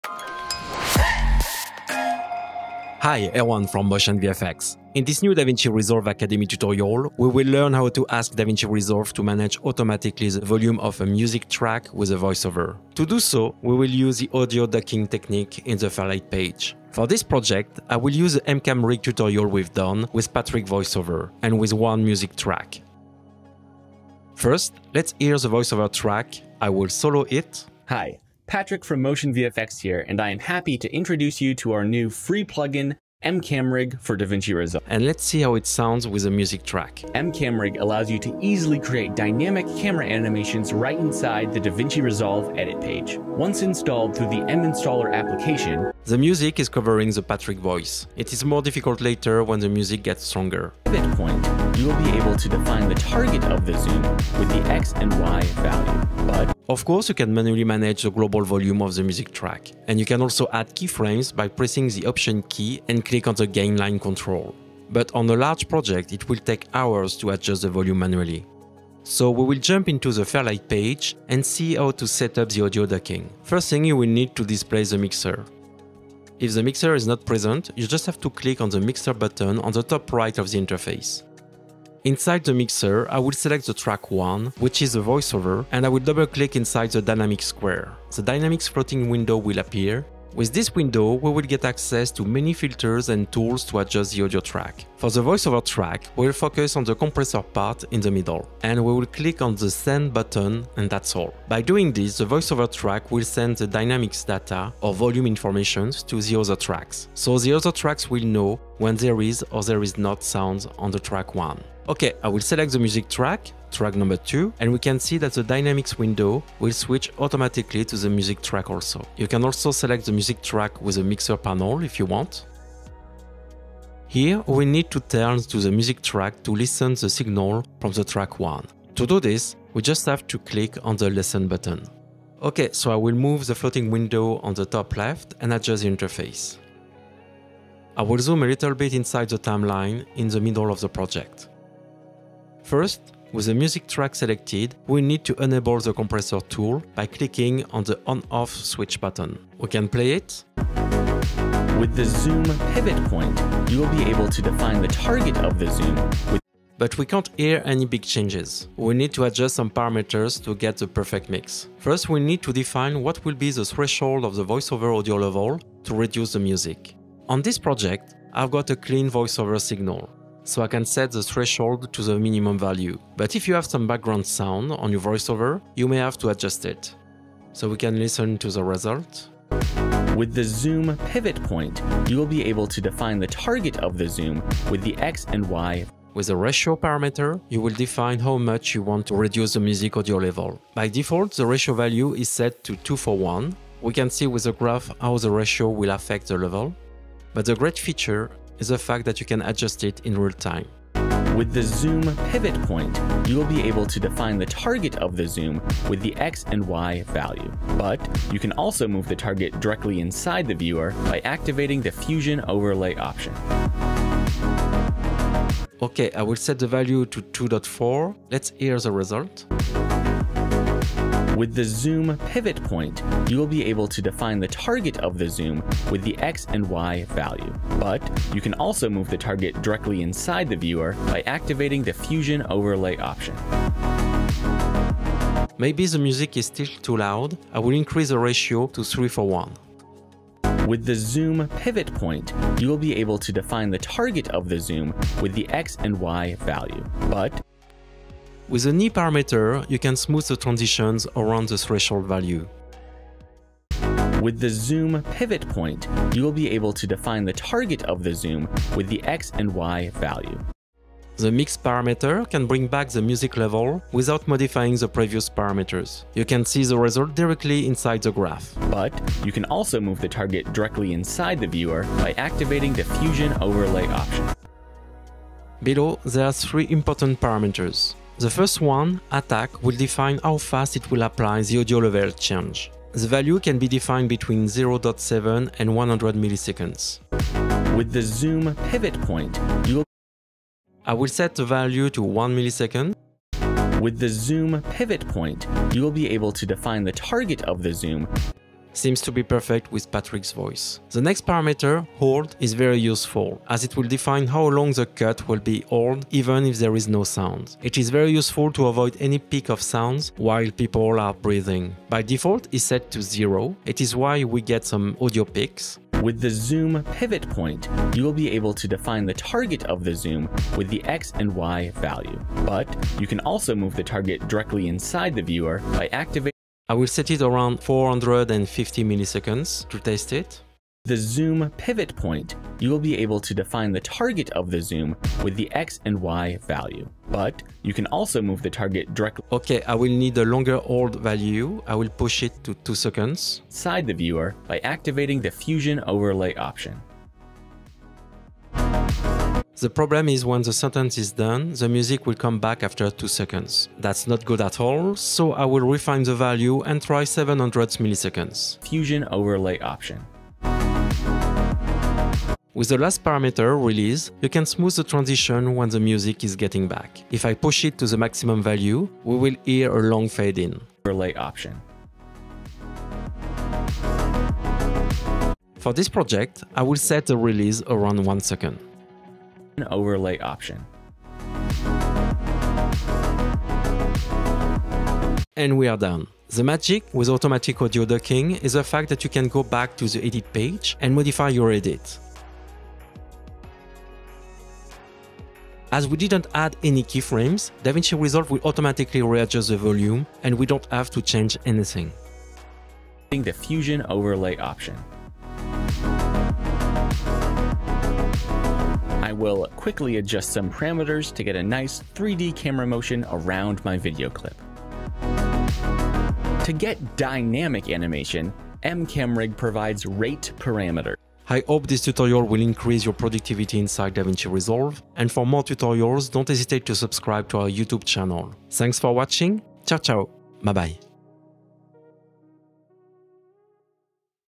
Automatic Audio Ducking — DaVinci Resolve Lesson 18 — MotionVFX Academy
In this DaVinci Resolve Academy Tutorial, Erwan Le Cloirec will teach us how to automatically manage the volume of a music with a voice over track. This technic is called “Audio Ducking” and it will be available inside the Fairlight page.

00: 00 Introduction
00:24 Analyzing the volume of the voice over and the music tracks
00:56 Manually adjusting the volume and adding keyframes
01:08 Switching to the Fairlight page
01:21 Opening the Dynamics controls window
01:35 Switching the voice over track to the “SEND” mode
02:04 Switching the music track to the “LISTEN” mode
02:22 Enabling the Compressor panel
02:36 Adjusting the Threshold parameter
03:06 Adjusting the Ratio parameter
04:08 Adjusting the Knee parameter
04:21 Adjusting the Mix parameter
04:35 Adjusting the Attack parameter
05:01 Adjusting the Hold parameter
06:02 Adjusting the Release parameter
06:26 Editing the project in the edit page
07:01 Conclusion